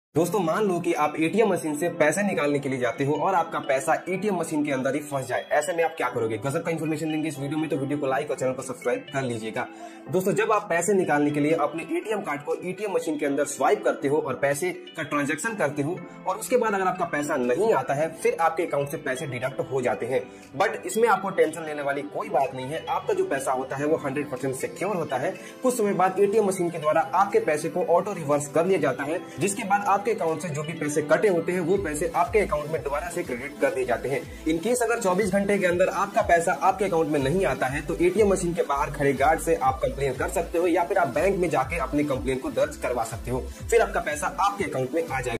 What to do if money gets stuck in the ATM machine?
* In case of money stuck in the ATM machine, you can also call the customer care immediately, but if you do not get a proper response from there, then you can go to your bank branch and lodge a complaint